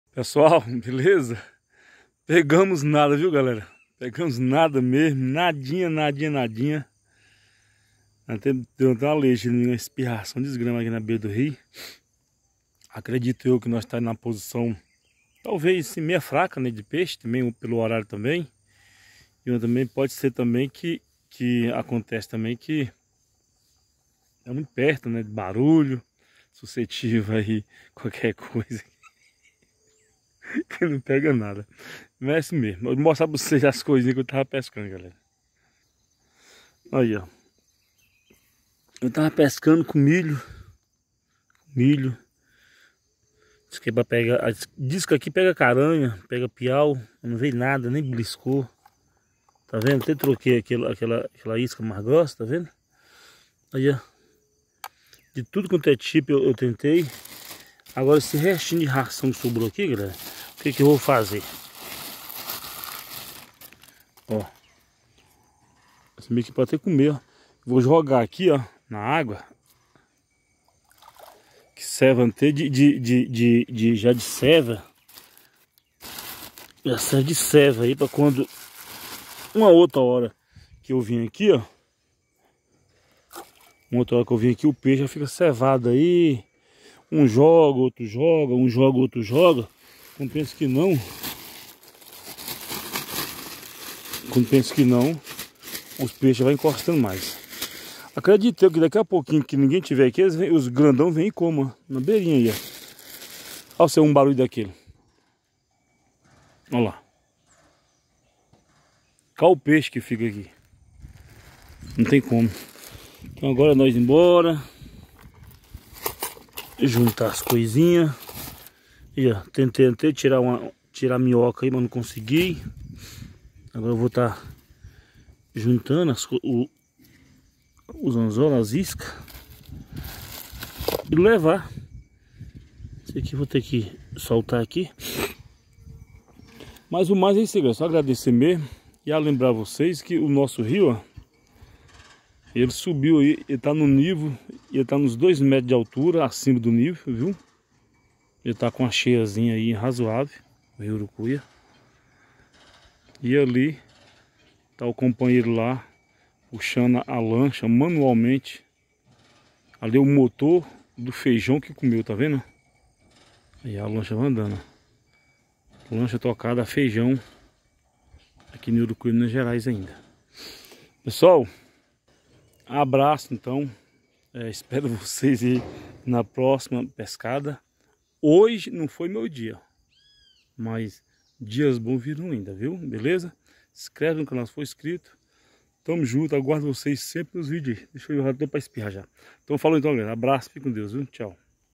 Fui pesca CARANHA, PIAUÇU e o que Aparecer. no Rio Urucuia em Urucuia-MG (3)
hoje foi o dia do peixe